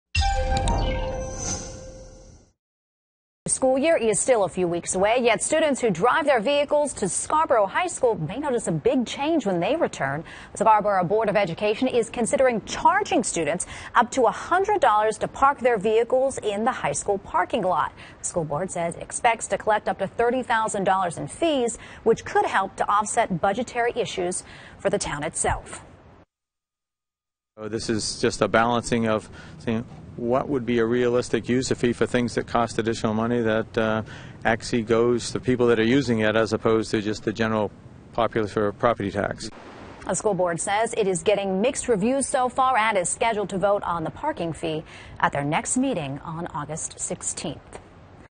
Scarborough considers high school parking fee
The start of the new school year is still a few weeks away, but students who drive their vehicles to Scarborough High School may notice a big change when they return.